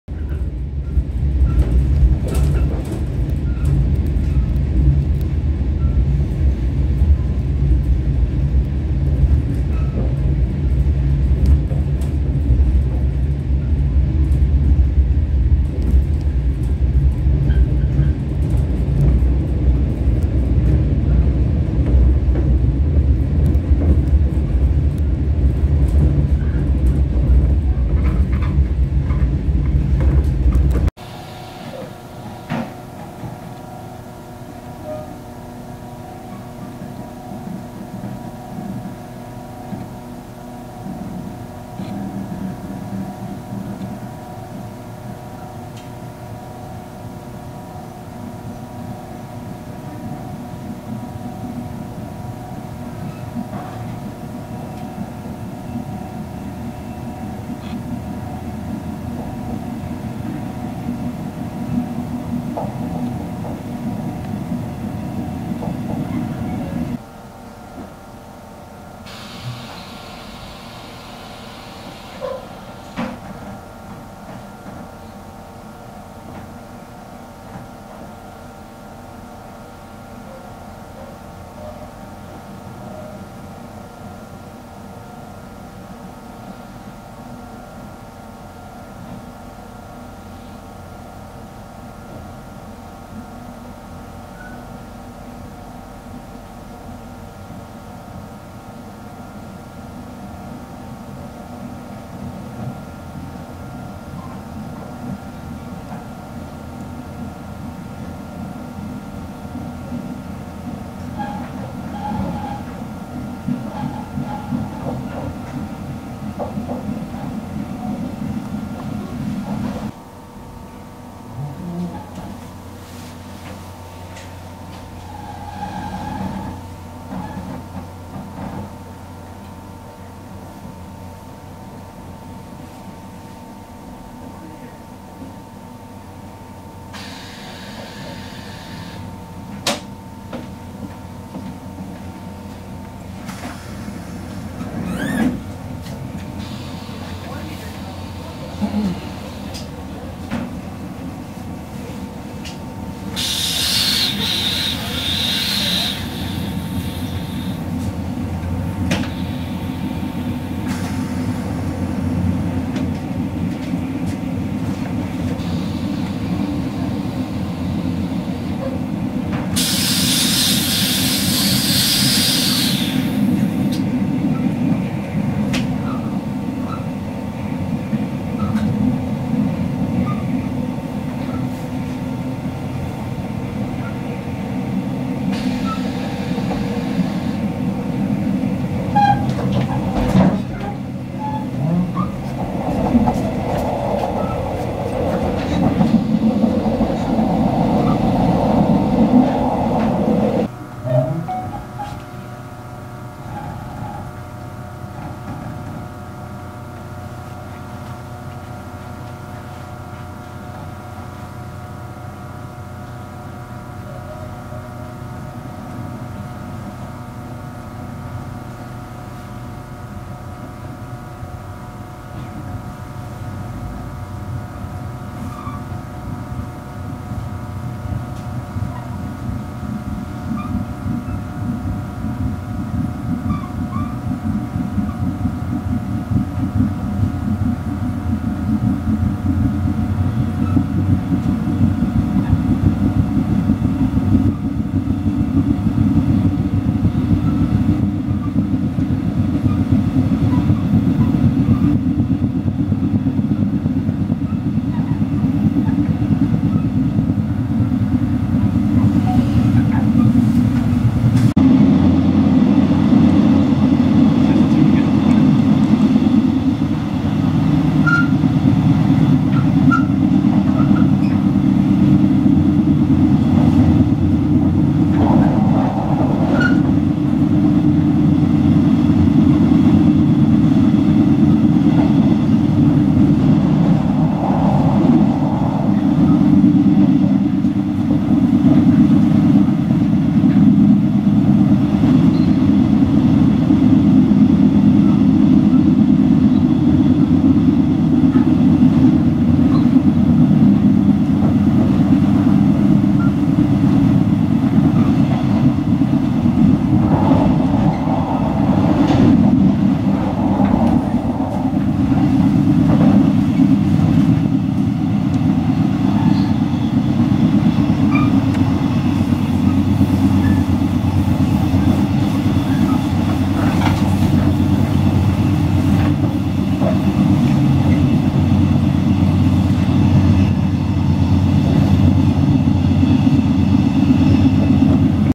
[철도주행] 한밤중의 동해선 경주~좌천 일주ㅣ1603열차 (2021.10.29)
[FHD 60FPS] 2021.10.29. 동해선 경주~좌천

무궁화호 제1603열차
청량리(14:50) → 부전(22:18)

00:00 경주 일대 주행
00:31 舊 불국사역 발차
01:07 舊 호계역 발차
02:00 태화강역 발차
03:21 덕하역 발차(임시승강장)
04:21 덕하~좌천 간 주행 중